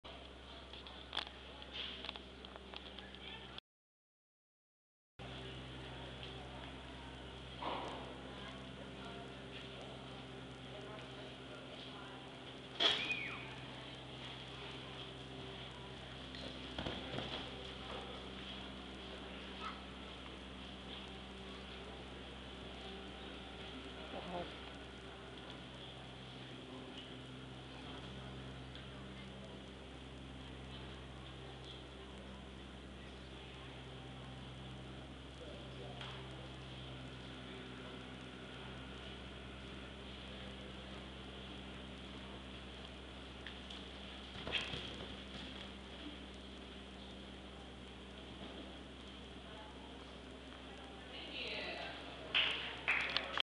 June 13, 2010 - Jumping Class
Twende's second time showing over fences. Where the camera goes off of us, he took the jump really long. We didn't place in our jumping classes, but I was really, really happy with how he did overall.